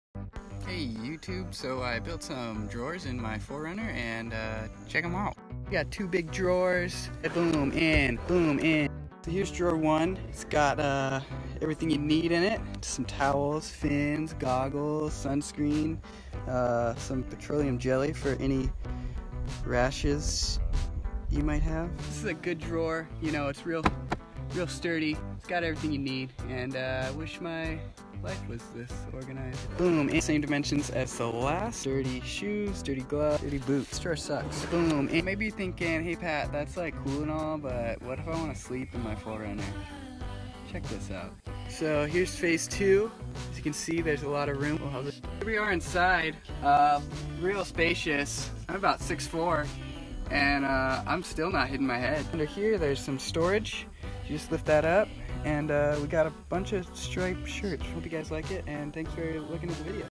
Toyota 4Runner Storage/ Sleeping Platform
Heres a brief video showing the storage and sleeping platform I built in my 2000 Toyota 4Runner. I love 3rd Gen 4Runners and seeing the modifications people add to theirs.